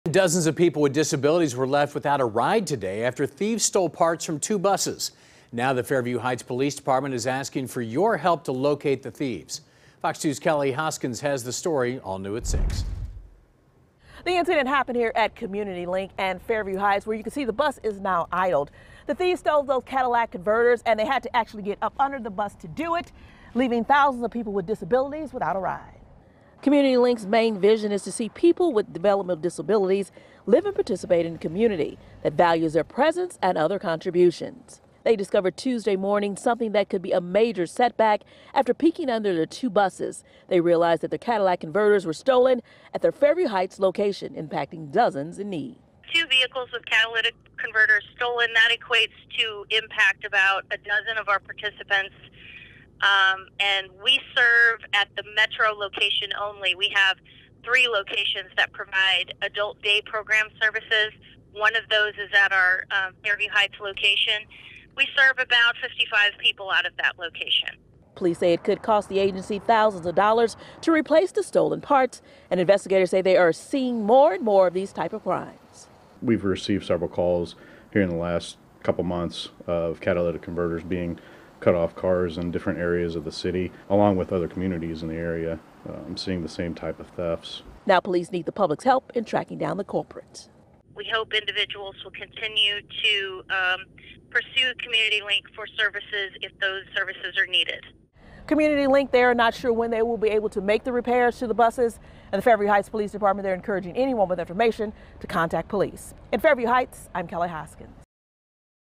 People with disabilities without rides after thieves steal parts from buses
Dozens of people with disabilities were left without a ride Tuesday after thieves stole parts from buses at a local agency. The Fairview Heights Police Department is asking for the public`s help to locate the thieves.